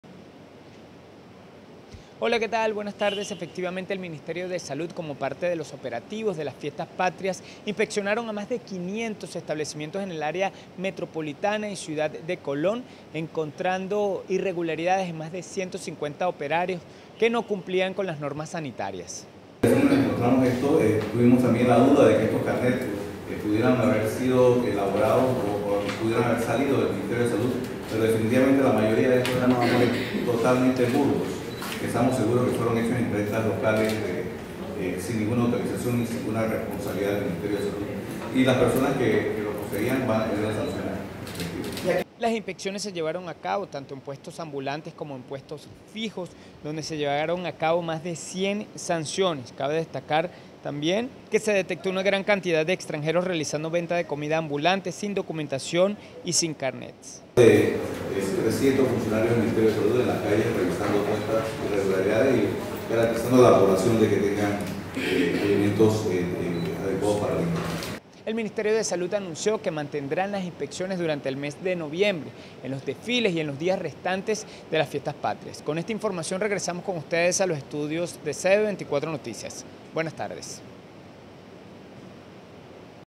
En Panamá: Más de 150 irregularidades se encontraron en permisos sanitarios en las fiestas patrias
El Ministerio de Salud realizó inspecciones a más de 500 establecimientos  fijos y ambulantes en la celebración de las fiestas patrias.

Ricardo Santana/Corresponsal Panamá/CB24